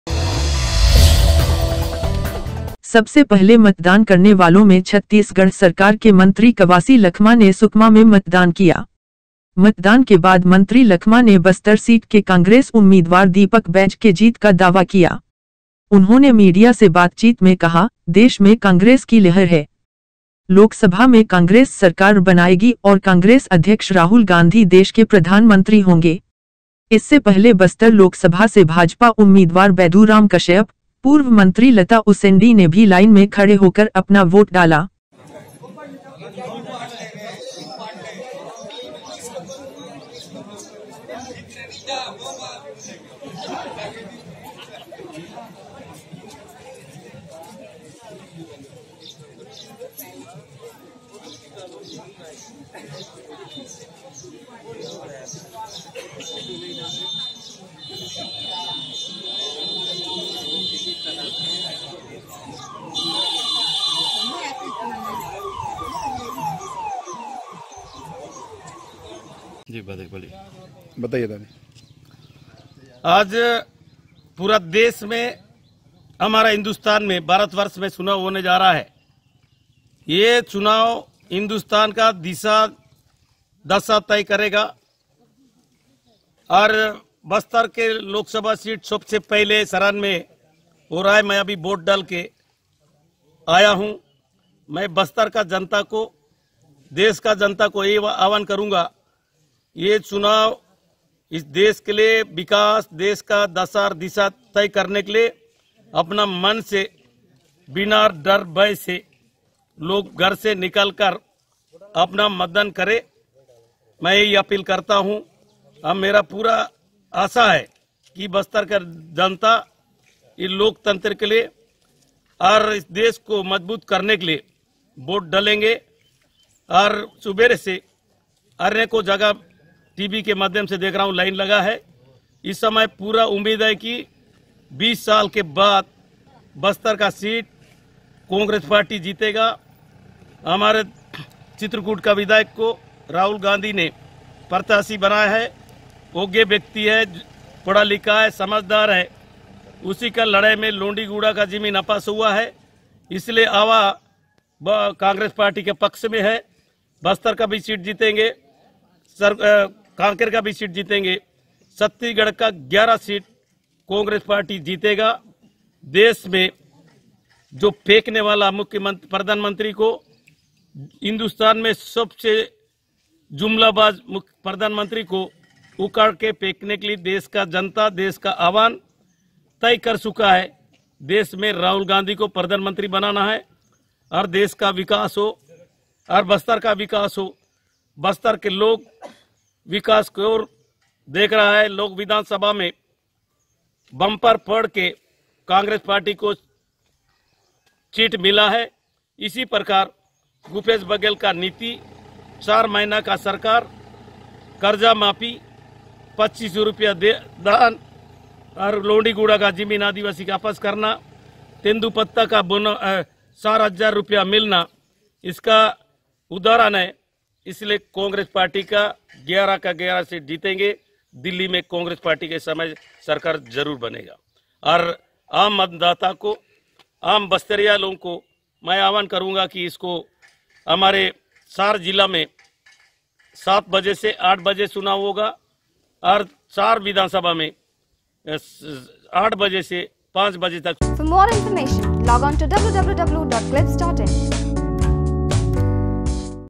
Exclusive : मंत्री कवासी लखमा ने किया मतदान
सबसे पहले मतदान करने वालों में छत्तीसगढ़ सरकार के मंत्री कवासी लखमा ने सुकमा में मतदान किया।